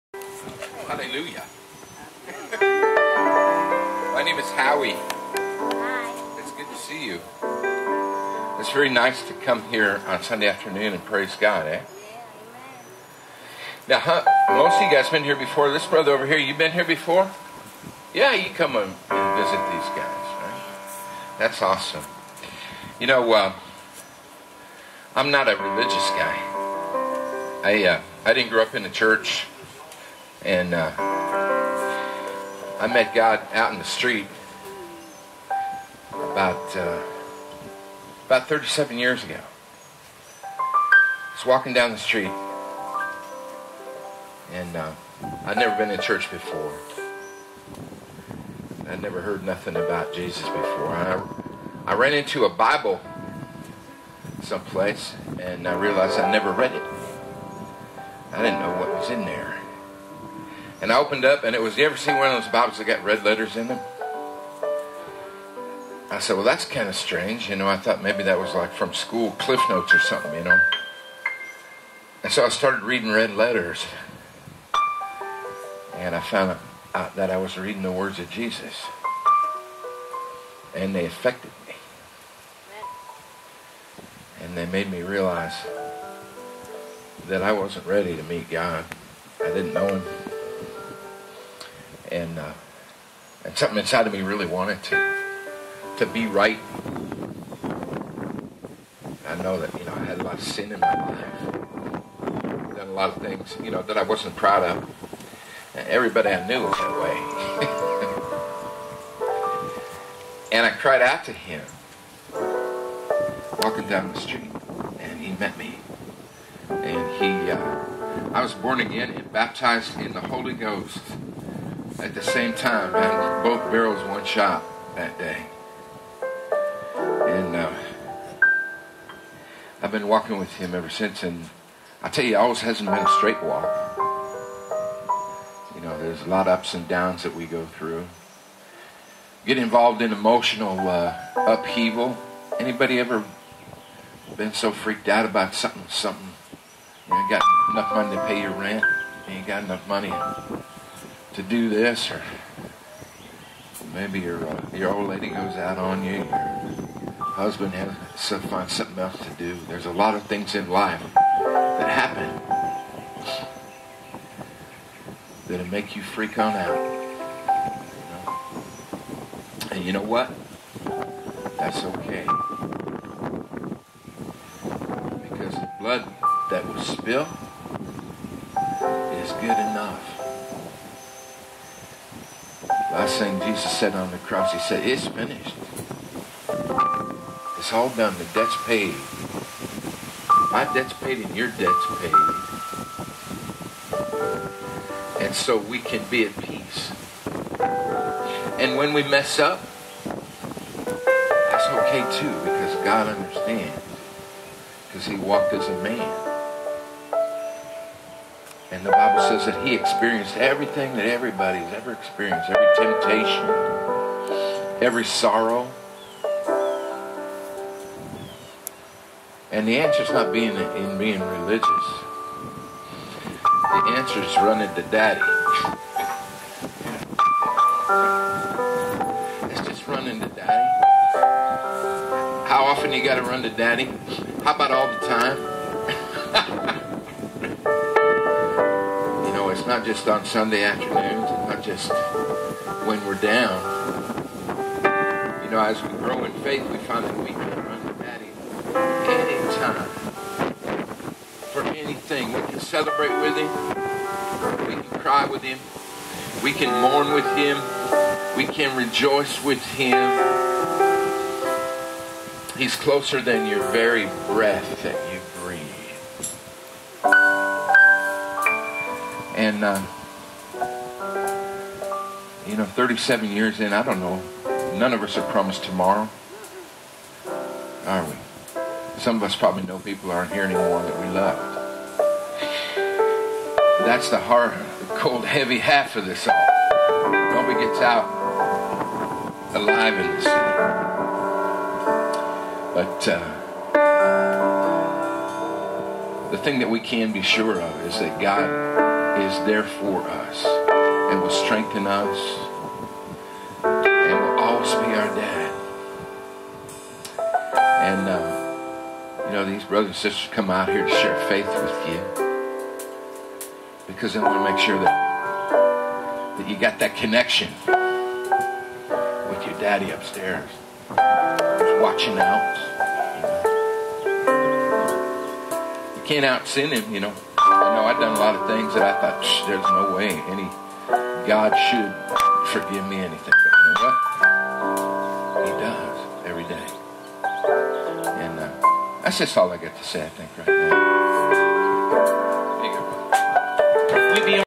Howie Bolton Tells How He Met Jesus/ st.cloud florida / / 5-1-11
Telling how he met Jesus